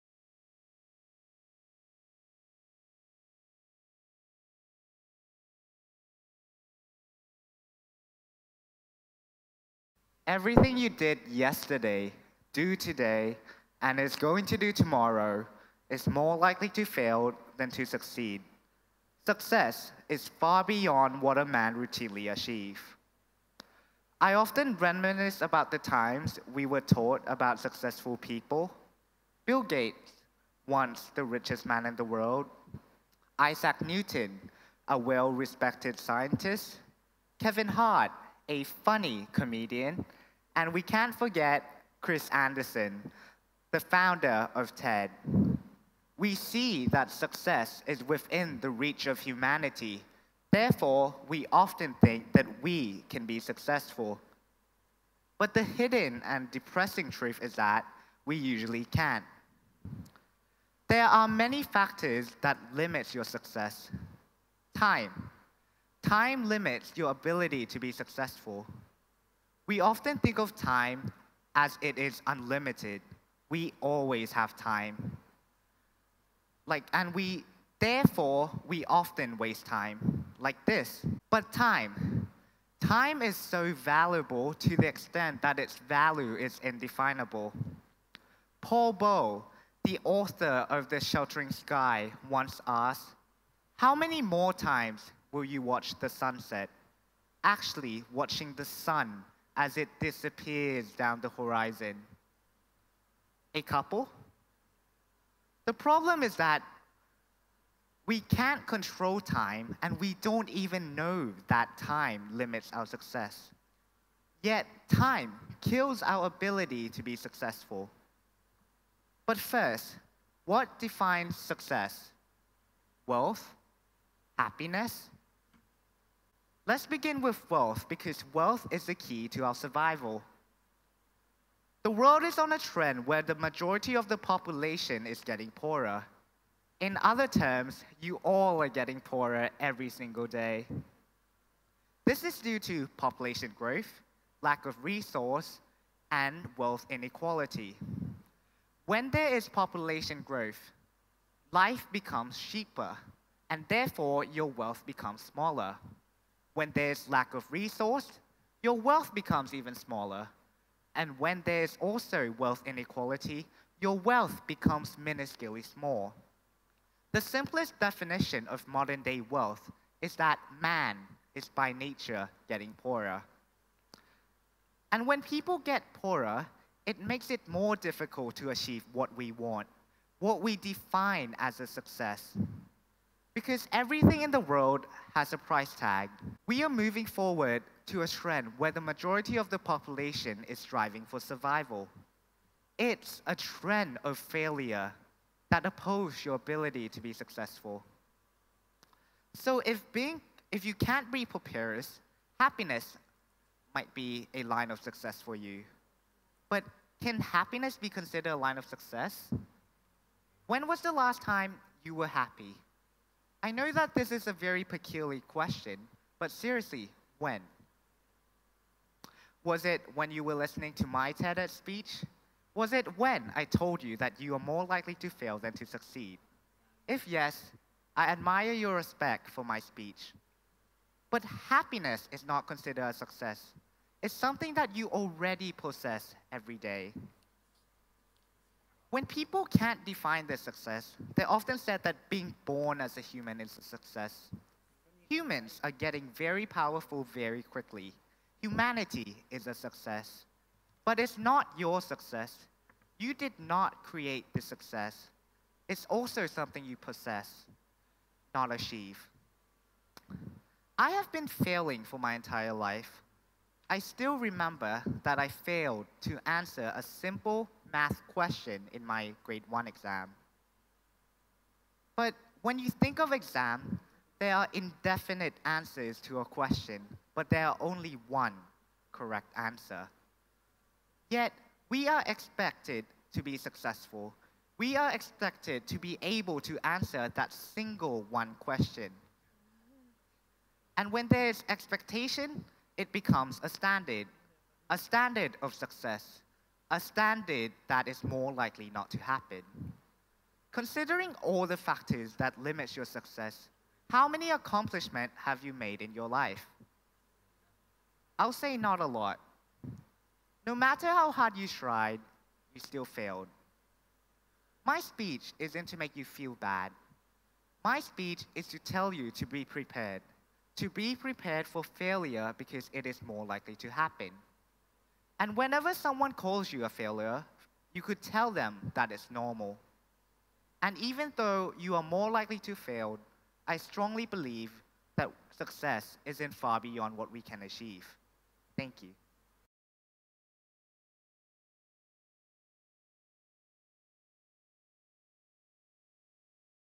Why you are more likely to fail than succeed | Iq Vanicharoenchai | KIS International School
Iq talks about the unrealistic expectations of success and how to prepare for the likelihood that most of us will fail.

Iq is a Grade 10 student from Thailand.